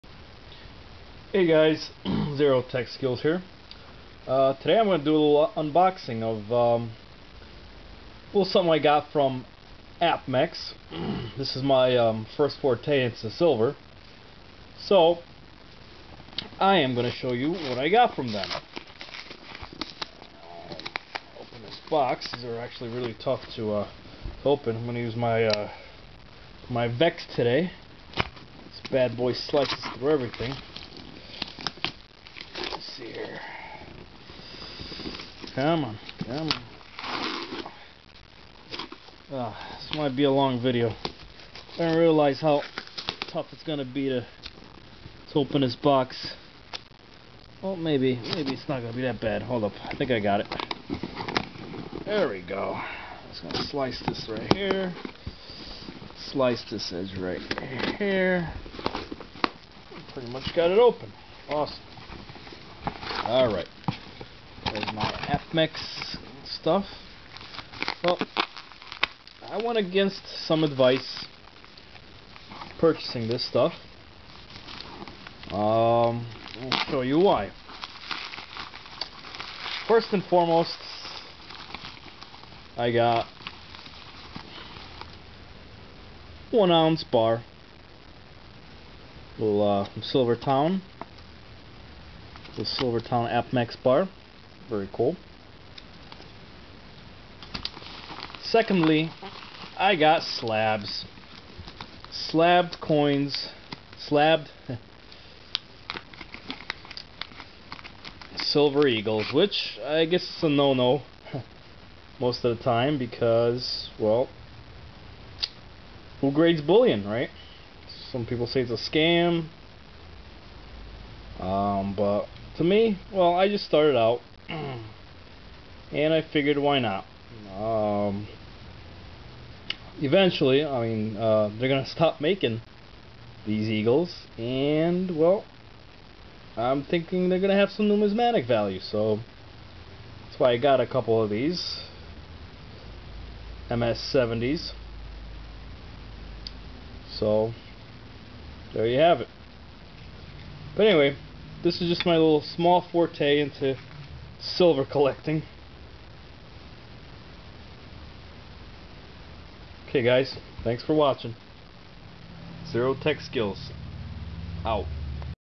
Apmex Silver Bullion Unboxing
My first silver bullion purchase.  I purchased two slabbed American Silver Eagles against the advice of coin/bullion experts but the price was right and I always wanted a perfect reference coin.  Also I picked up a small 1oz. bar just so as to get my feet wet.